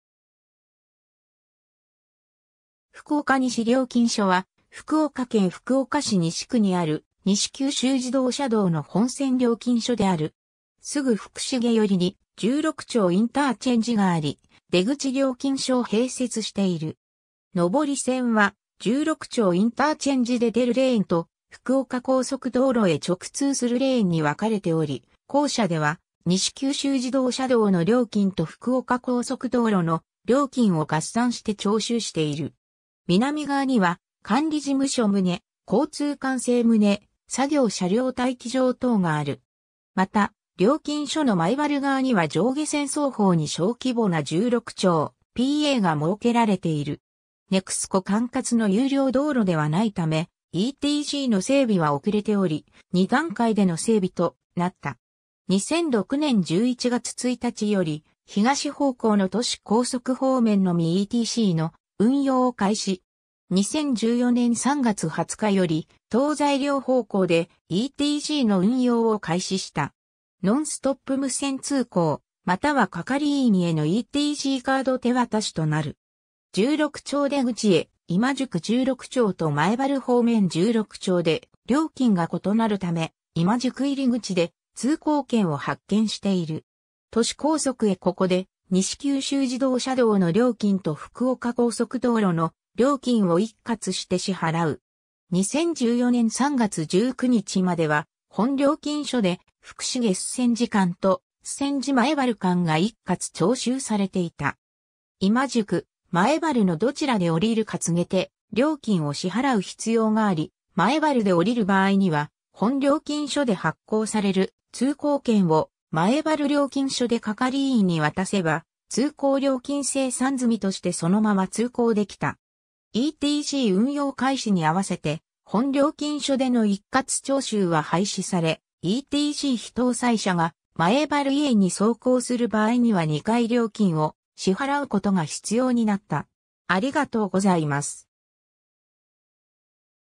福岡西料金所
福岡西料金所（ふくおかにしりょうきんじょ）は福岡県福岡市西区にある、西九州自動車道（福岡前原道路）の本線料金所である。
 すぐ福重寄りに拾六町ICがあり、出口料金所を併設している（入口は前原方面と共通）。
上り線は拾六町ICで出るレーンと福岡高速道路へ直通するレーンに分かれており、後者では西九州自動車道（福岡前原道路）の料金（周船寺IC - 福岡西料金所分）と福岡高速道路の料金を合算して徴収している。
 南側には管理事務所棟・交通管制棟・作業車両待機場等がある。
また、料金所の前原側には上下線双方に小規模な拾六町PA（トイレと自動販売機が設置されている）が設けられている。
　　 NEXCO管轄の有料道路ではないため、ETCの整備は遅れており、2段階での整備となった。
 2006年11月1日より東方向の都市高速（福岡高速道路）方面のみETCの運用を開始。
2014年3月20日より東西両方向でETCの運用を開始した。
ノンストップ無線通行または係員へのETCカード手渡しとなる。
 拾六町出口へ (東方向) 今宿→拾六町と前原方面→拾六町で料金が異なるため、今宿入口で通行券を発券している。
 都市高速へ (東方向) ここで西九州自動車道（福岡前原道路）の料金（今宿IC - 福岡西料金所分）と福岡高速道路の料金を一括して支払う。
 2014年3月19日までは本料金所で福重-周船寺間（普通車 : 150円）と周船寺-前原間（普通車 : 200円）が一括徴収されていた。
今宿、前原のどちらで下りるか告げて料金を支払う必要があり、前原で下りる場合には本料金所で発行される通行券を前原料金所で係員に渡せば通行料金精算済みとしてそのまま通行できた。
ETC運用開始に合わせて本料金所での一括徴収は廃止され、ETC非搭載車が前原以遠に走行する場合には2回料金を支払うことが必要になった。